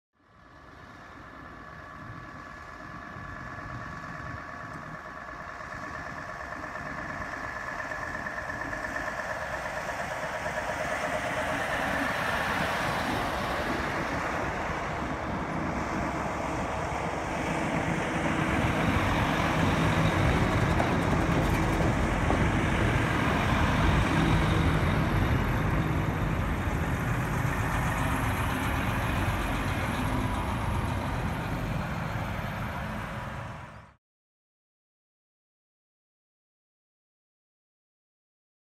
Arrival of new props at Delta Force Paintball
Our new props including Jaguar fighter jets and Lynx aircraft arriving for installation at selected Delta Force Paintball centres around the UK.